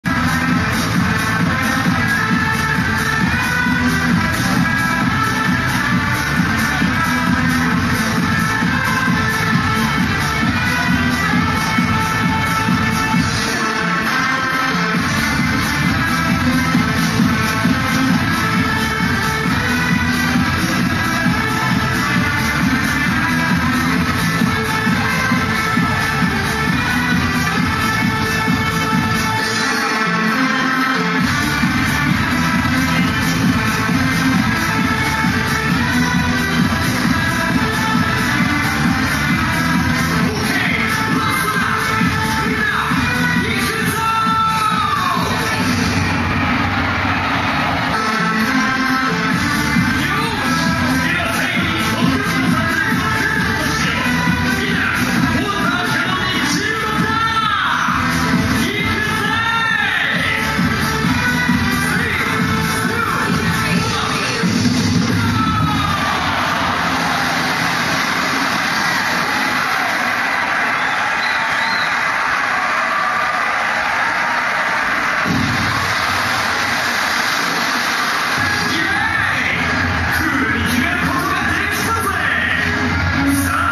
USJ 水パーティー 凄い水量ですｗ
2011年7月17日

ユニバ、水パーティー
水、凄過ぎです！！